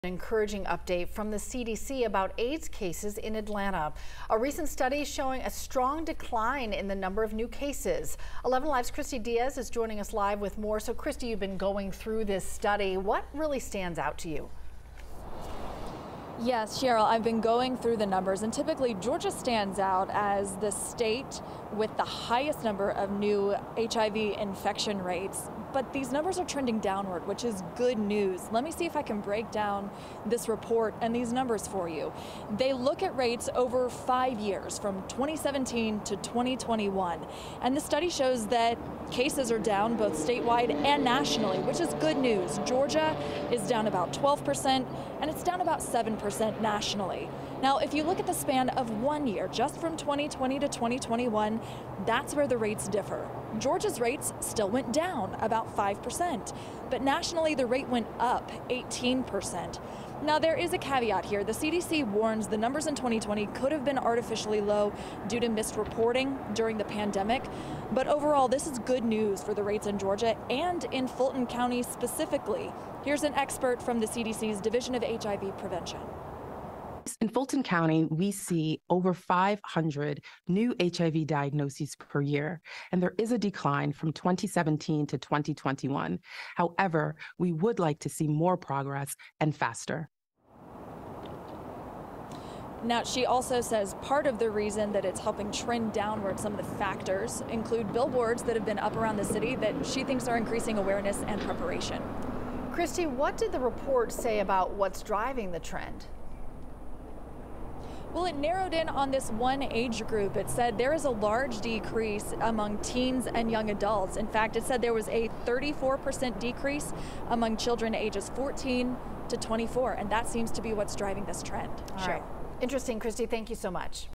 New CDC data shows rate of HIV spread in Georgia continuing to slow
Georgia has long had one of the highest rates in the nation, though it bucked a trend in 2021 as its new diagnoses decreased even as they increased nationally.